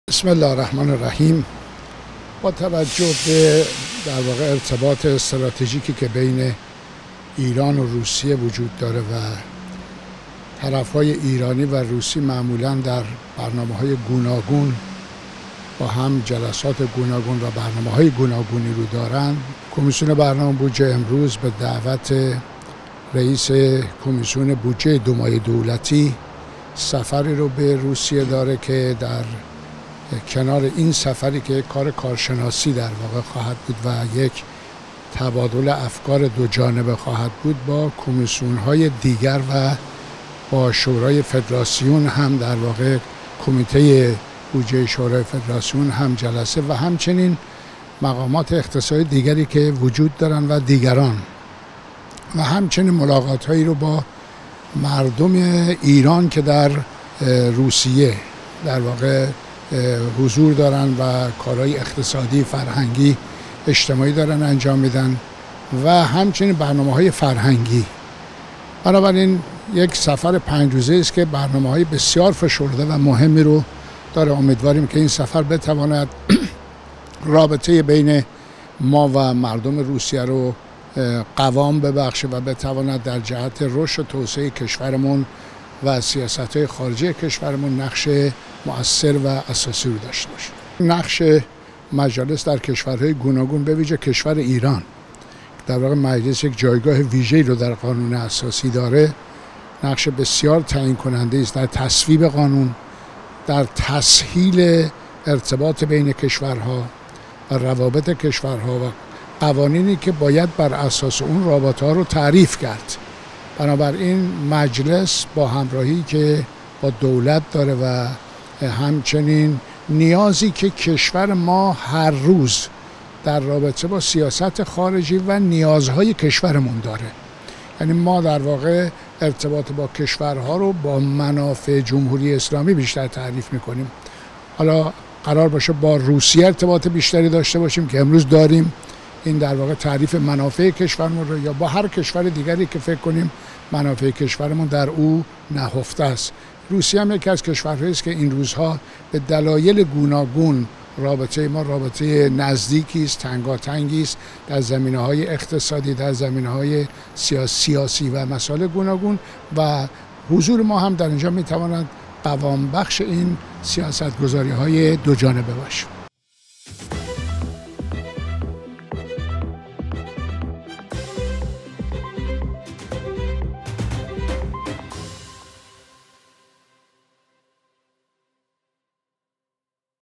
Iranian parliamentary delegation arrives in Moscow
The Iranian parliamentary delegation consisting of the chairman and several members of the Program, and Budget Commission of the Iranian Parliament has traveled to Moscow to meet with members of the State Duma and the Council of the Russian Federation.